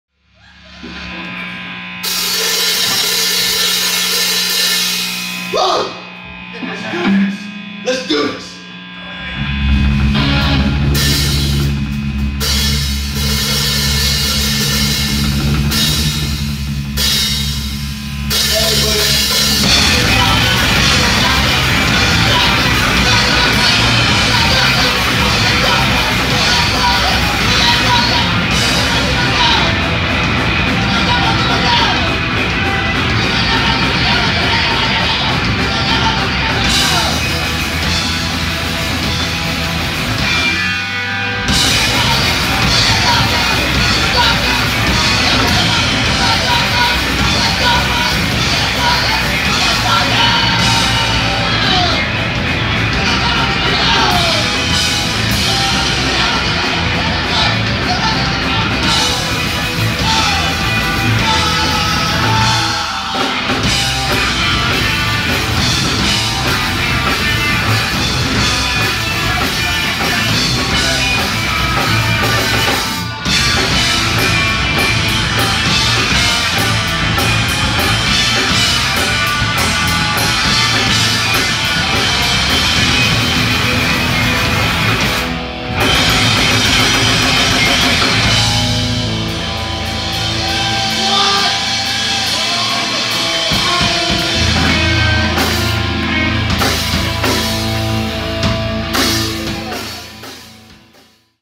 Blue Ohms @ Romano's in Riverside, CA 1-15-2018
Date: Tuesday January 15, 2018
City: Riverside
State: California
Venue: Romano's
Other band(s):  Quits, Ball Of Light, The Lungs